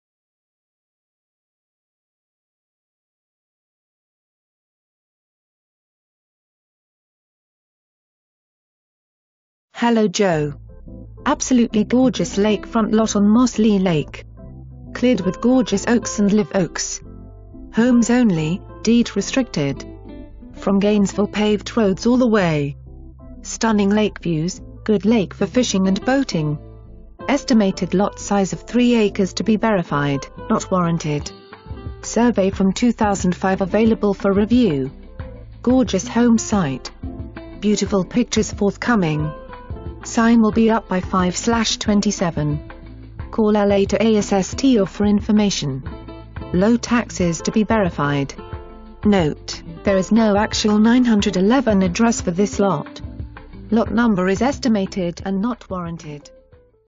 Homes for Sale - 24739 SE 101st Avenue, Hawthorne, FL
Absolutely gorgeous lakefront lot on Moss Lee Lake . Cleared with gorgeous oaks and live oaks. Homes only, deed restricted. From Gainesville paved roads all the way! Stunning lake views, good lake for fishing and boating. Estimated lot size of 3 acres to be verified, not warranted. Survey from 2005 available for review. Gorgeous home site. Beautiful pictures forthcoming. Sign will be up by 5/27. Call LA to asst or for information. Low taxes to be verified. Note: There is no actual 911 address for this lot. Lot number is estimated and not warranted. Seller will consider owner financing, too.

Presented By:
Joseph Freck, Berkshire Hathaway HomeServices Florida Realty